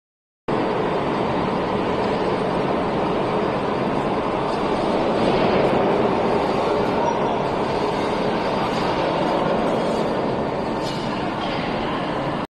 CNC rotor milling machine (four axis) processing vacuum pump roto, sent to Zhejiang......
CNC rotor milling machine (four axis) processing vacuum pump rotor, sent to Zhejiang.